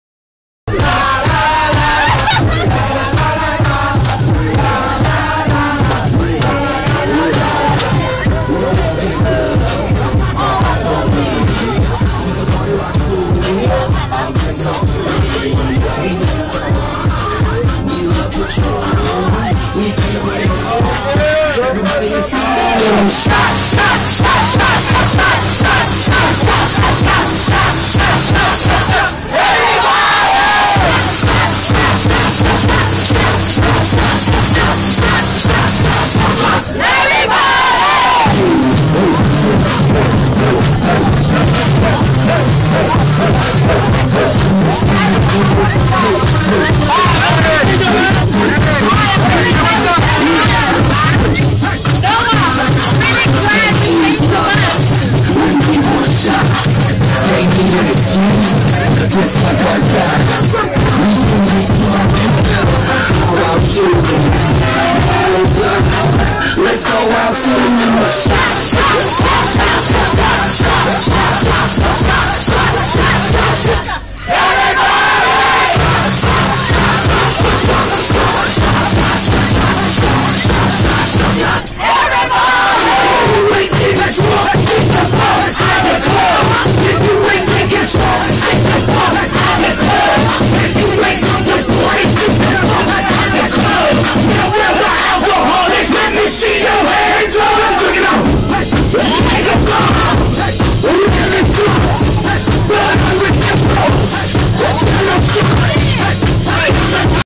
The Ultimate Party Bus
What do you get when you mix a tour bus, 40 ppl from the Philippines, Canada, USA, and South Korea??